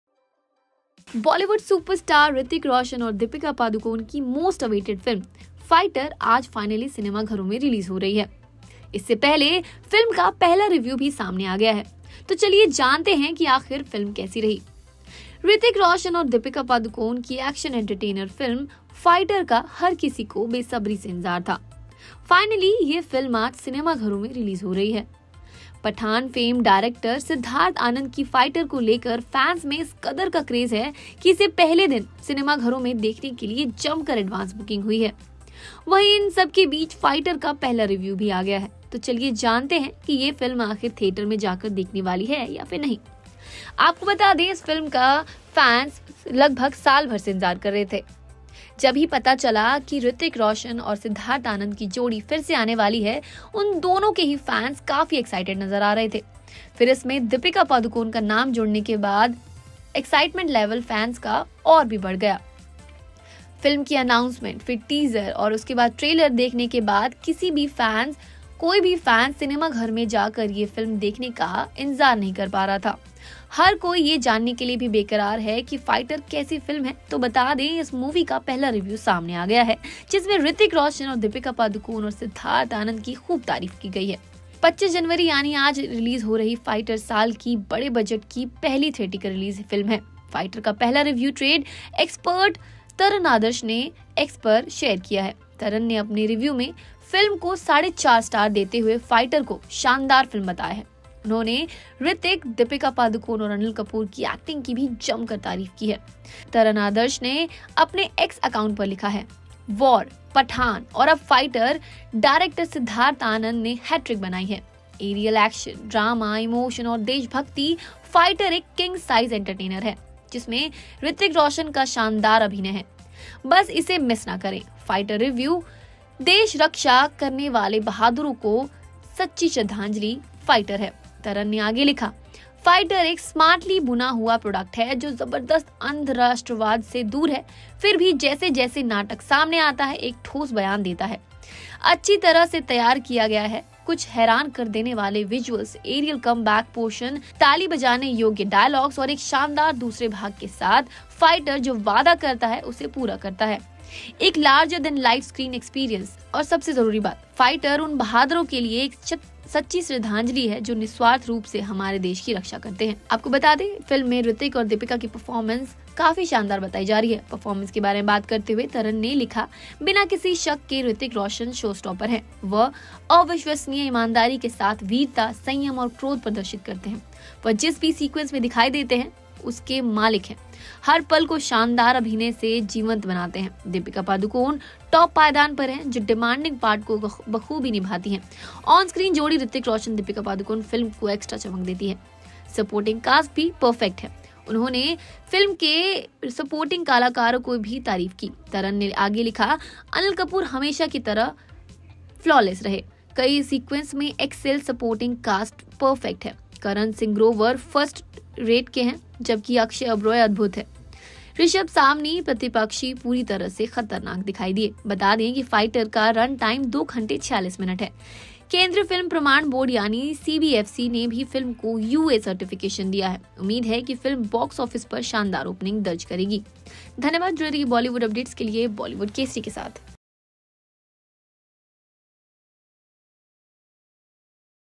Fighter Review: सिनेमाघरों में रिलीज हुई फाइटर, फैंस का आया सोशल मीडिया रिव्यू
Hrithik Roshan and Deepika Padukone's most awaited film 'Fighter' is releasing in theaters today. Before this, the first review of the film has also arrived. Let us know how is this film?